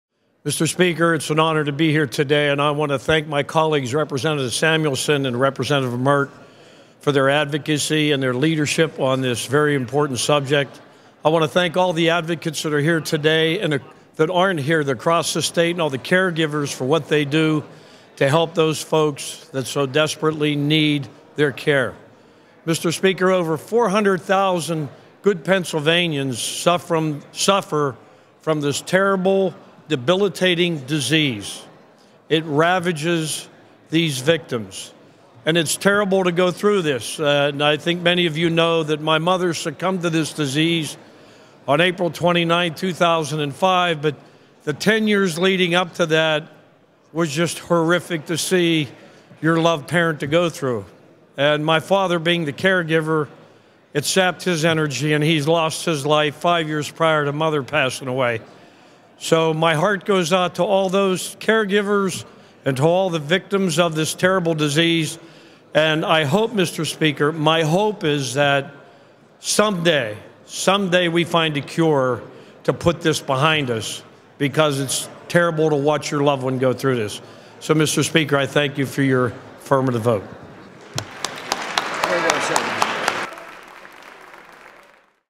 Kortz Speaks Out About Alzheimer's Disease
Pa. state Rep. Bill Kortz speaks on a bill designating November as Alzheimer's Disease Awareness and Family Caregivers Month. Kortz has been personally affected by this disease and wanted to recognize the families and caregivers.